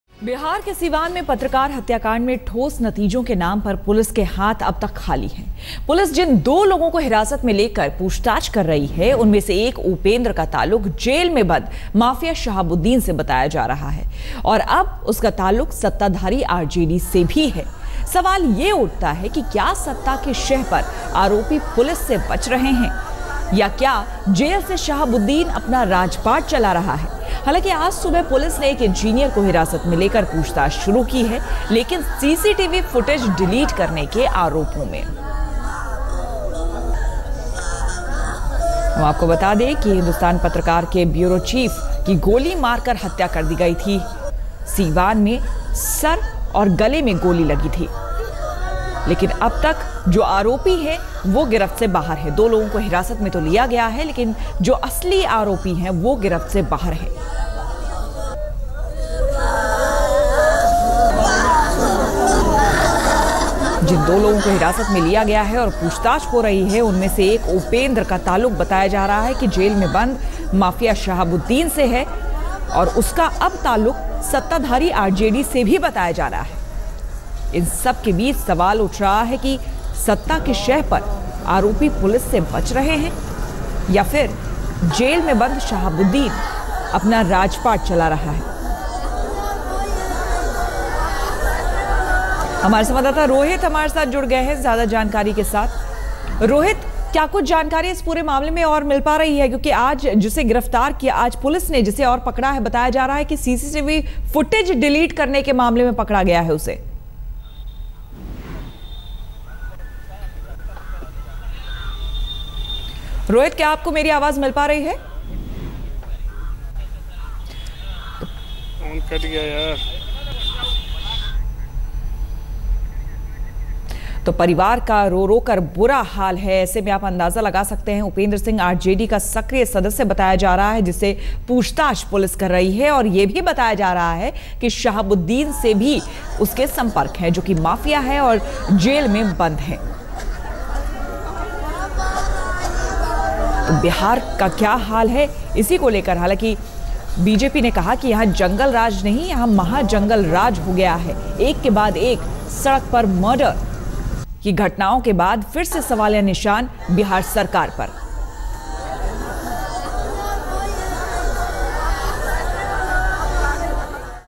Bihar Police Yet To Get Strong Evidence In Journalist's Murder
The Bihar Police is yet to find a strong evidence in the murder of journalist Rajdev Ranjan. One of the suspects Upendra Kumar is closely linked to gangster Mohammed Shahabuddin.